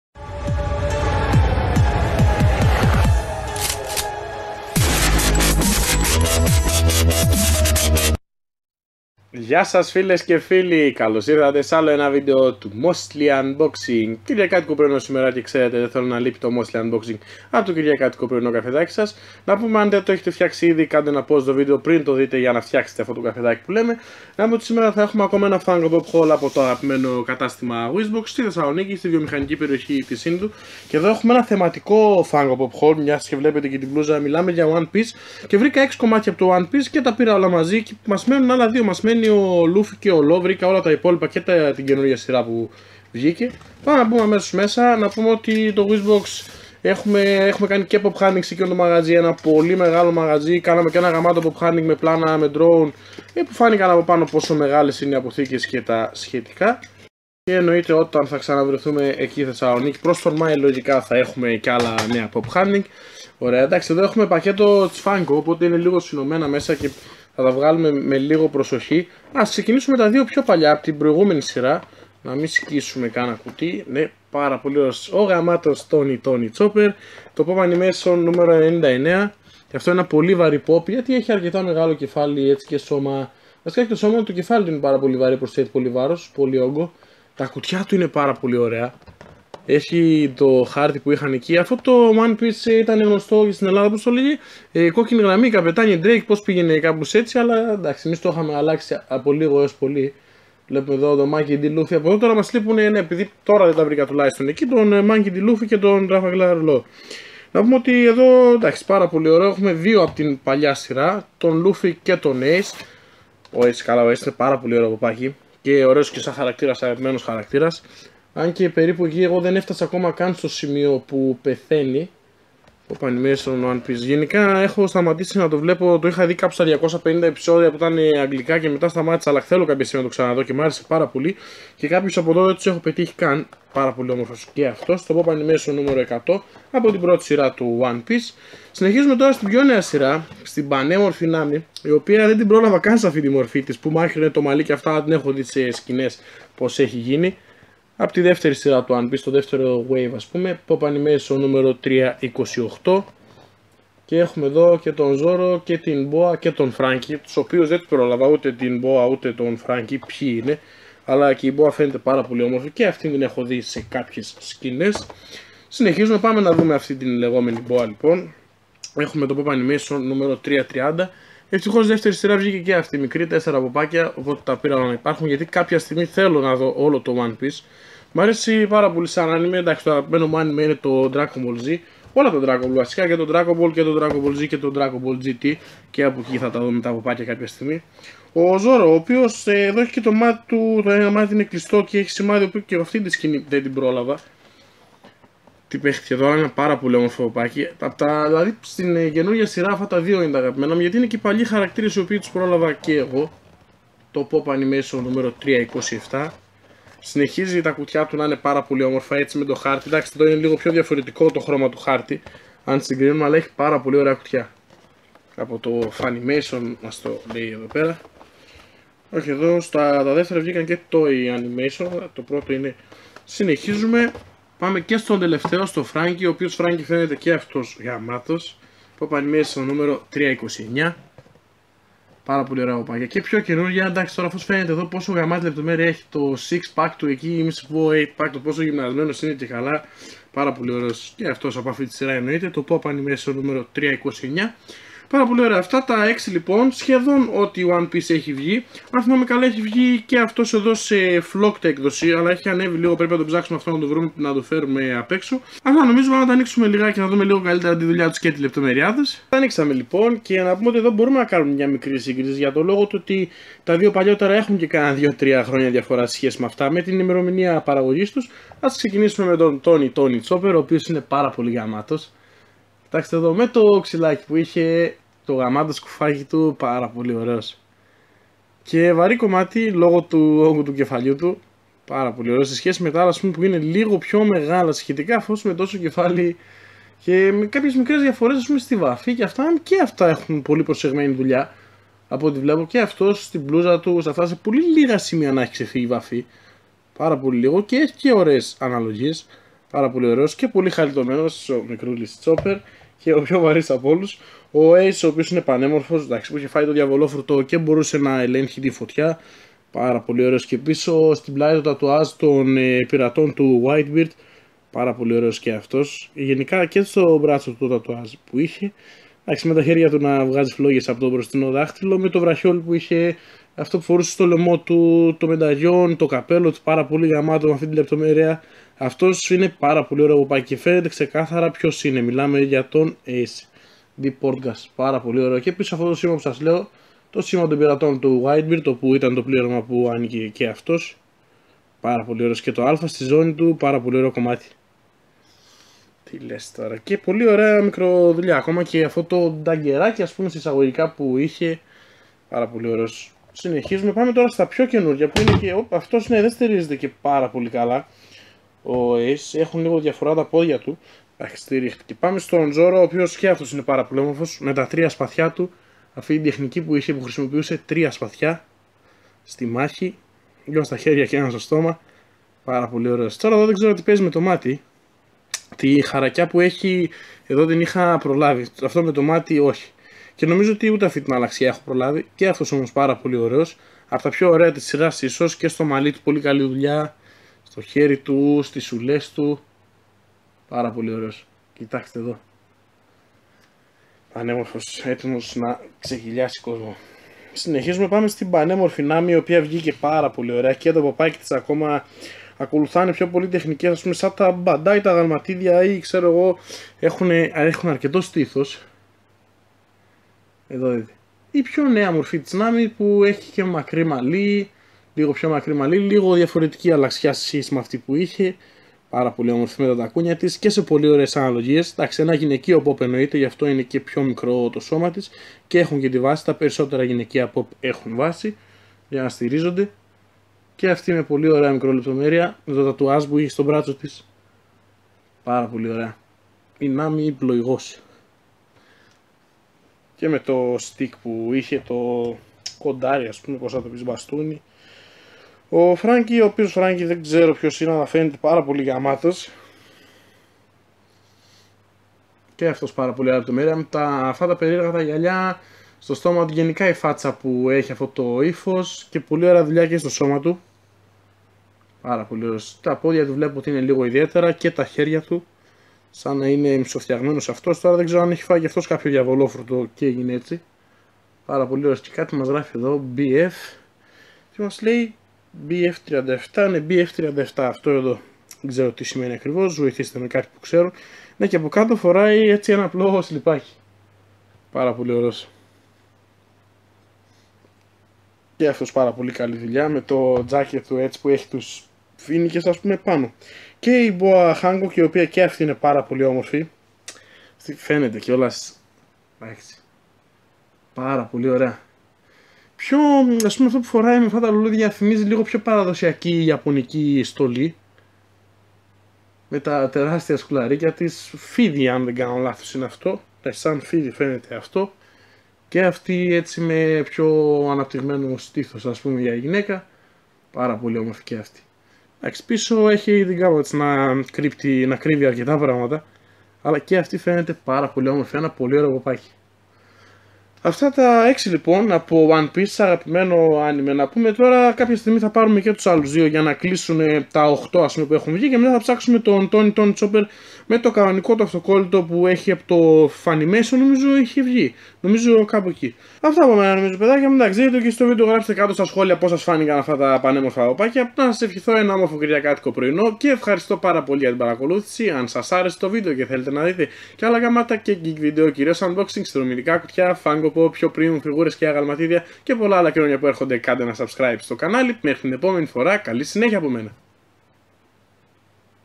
WIZBOX FUNKO POP! Haul #27 | Καλό μήνα και καλές Διακοπές!
Σήμερα ανοίγουμε πακέτο από Wizbox με 6 κομμάτια από το αγαπημένο Anime,One Piece!